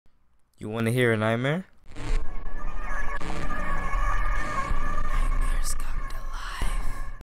Mod3rn N1ghtmar3 intro
A new scary story series I'll be doing

mod3rn n1ghtmar3
scary story